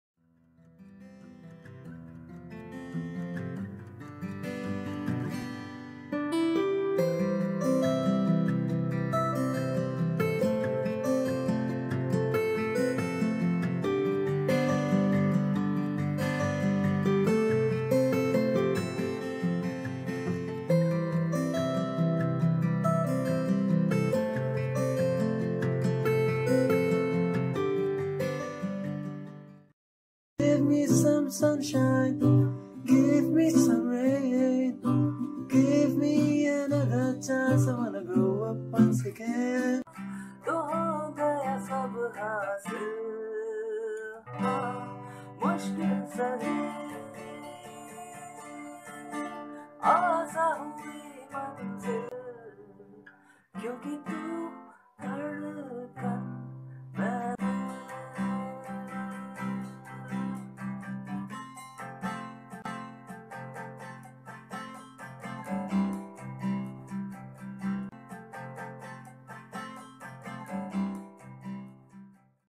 Teachers' Day Celebrations - 2020
We would have loved to be on campus to celebrate today. But nonetheless we celebrated our first virtual teachers' day with the same zeal.